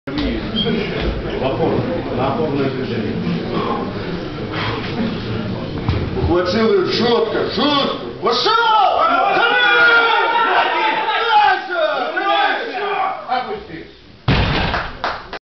Cham BLR powerlifting 2006 ##
Чемпионат Беларуси по пауэрлифтингу
место проведения г. Брест, 2006 г.